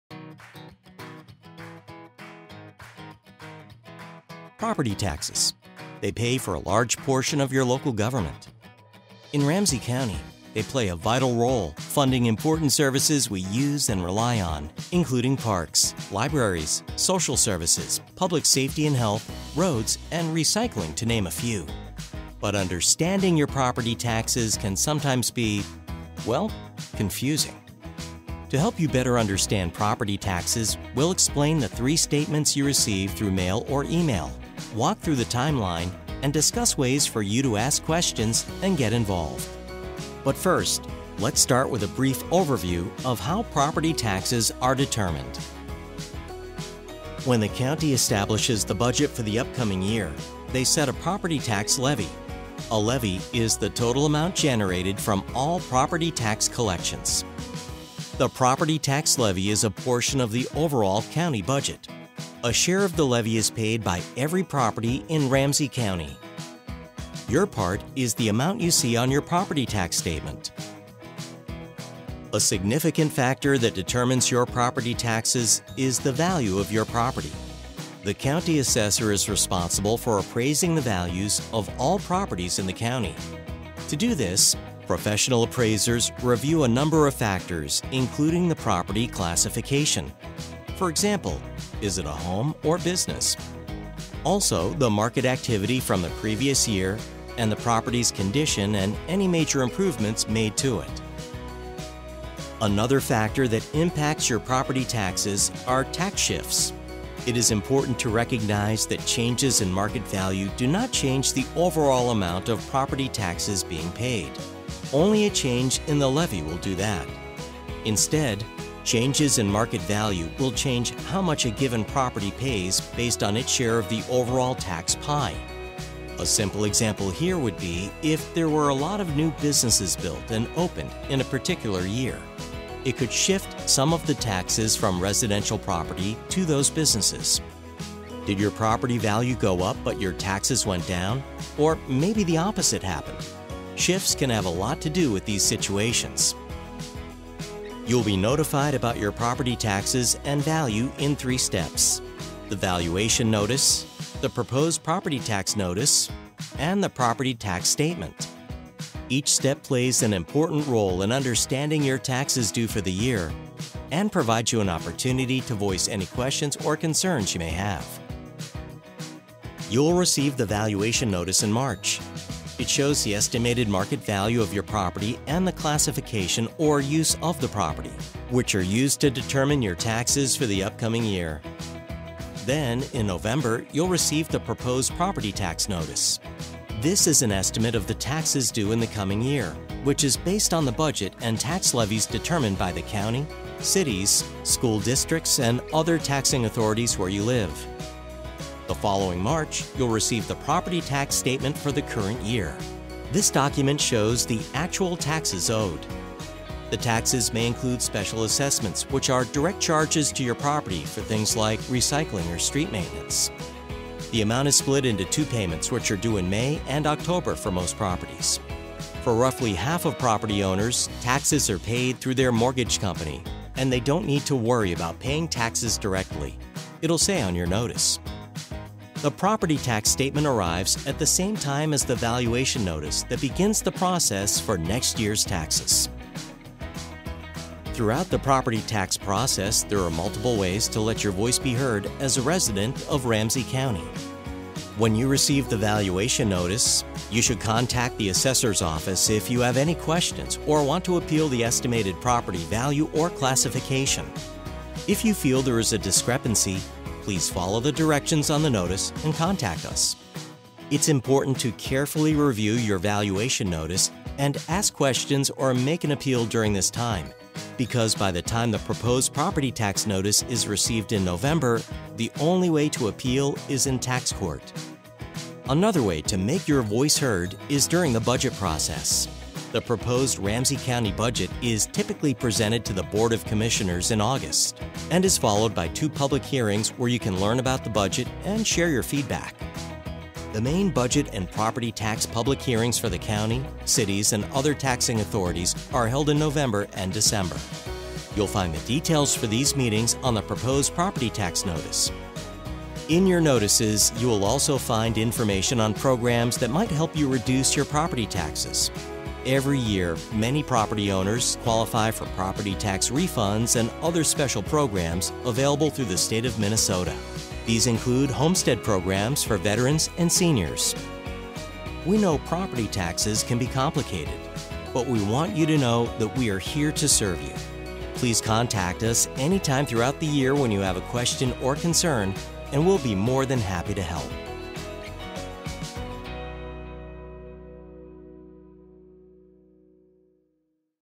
Understanding Your Property Taxes and Values
Watch this video to learn more about how taxes are determined, how to read your statement and how to resolve questions.